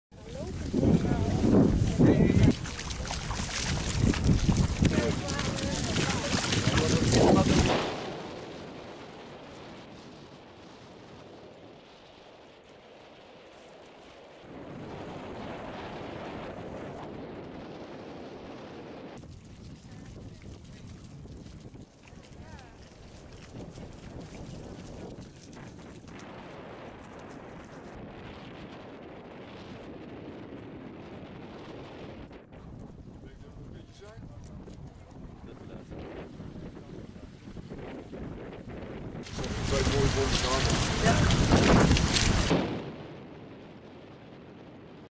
Wadlopen bij Paesens-Moddergat
Wandeling tussen de pier bij Paesens-Moddergat en de vaargeul de Paesensrede.